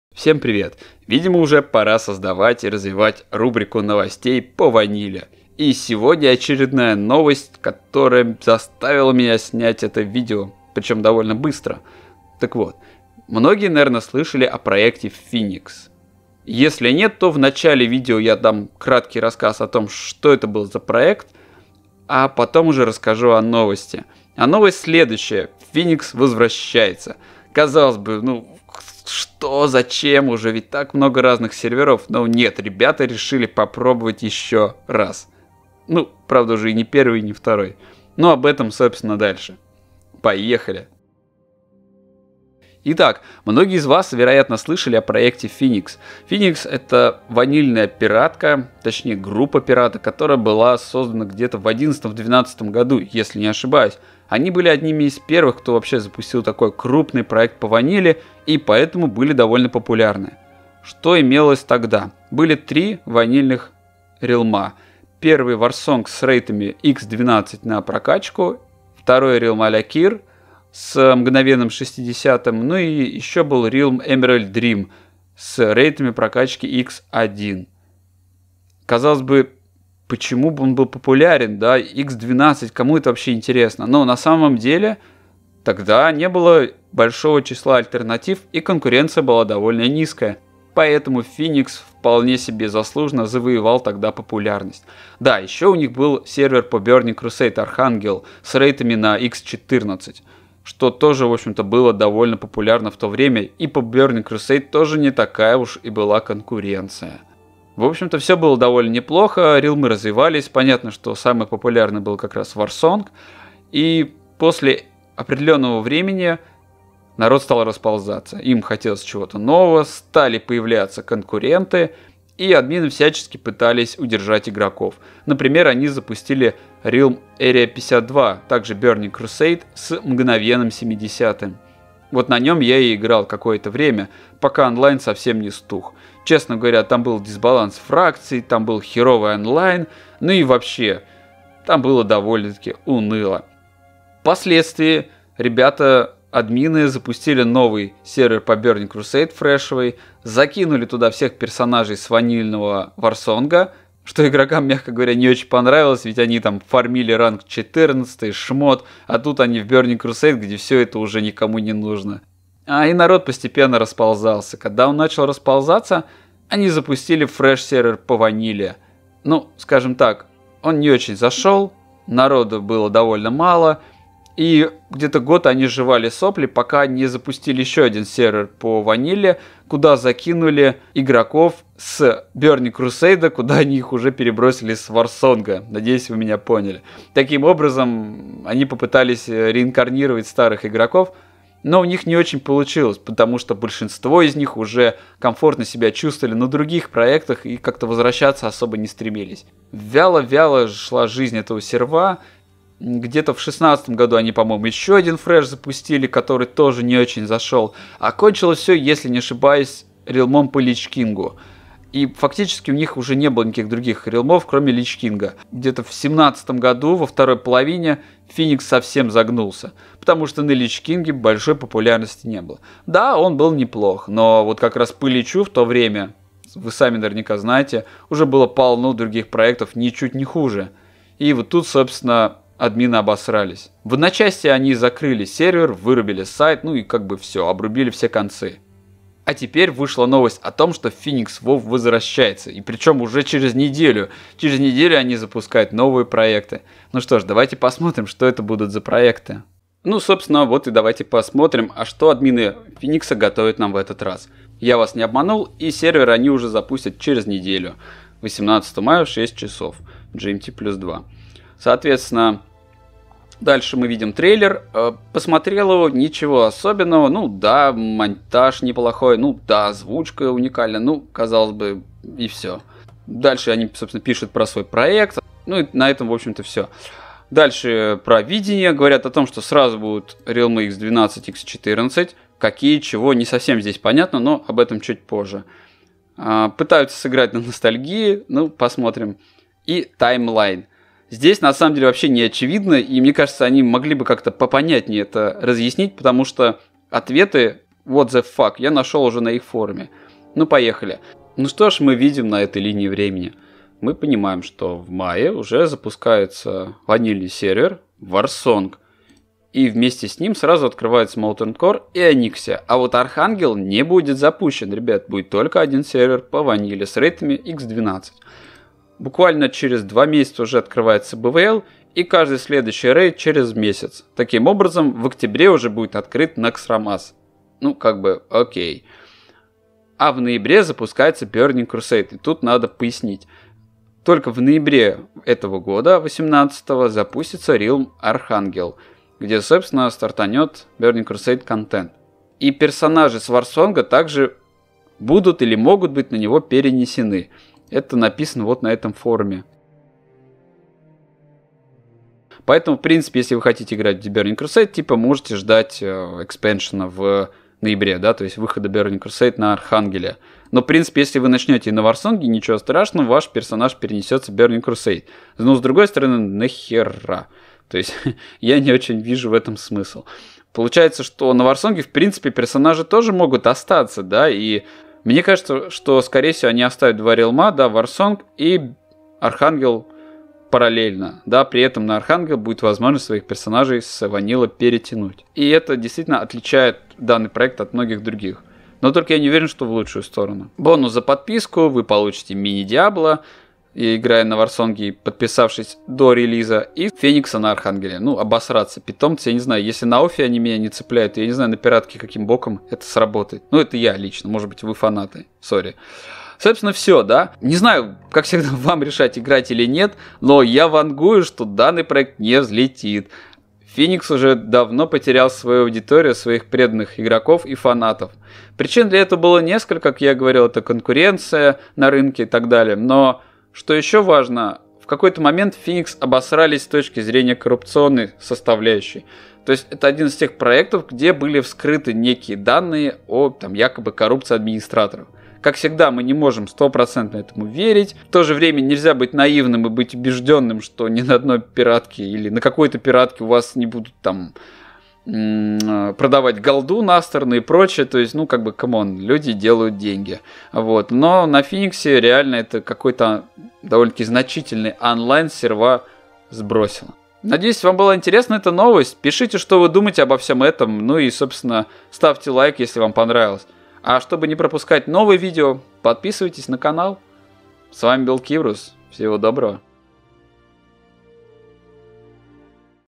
WoW Classic: Feenix WoW возвращается!? Обзор и мнение
На днях узнал что Feenix WoW возвращается!  В этом видео расскажу краткую историю сервера Feenix, а также что он нам готовит в очередном своем перезапуске.

P.S.S. я сам играл на фениксе только в БК и не очень долго. Так что если услышите какие-то неточности - можете написать в комментариях